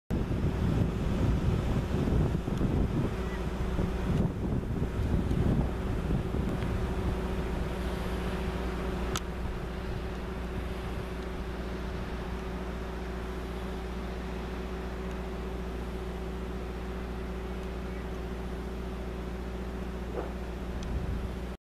OGGI A CAPRI 19/02/2016 METEO REPORTER
Situazione Meteo a Capri del 19/02/2016 zona dei Faraglioni, in collaborazione con Caprinotizie.it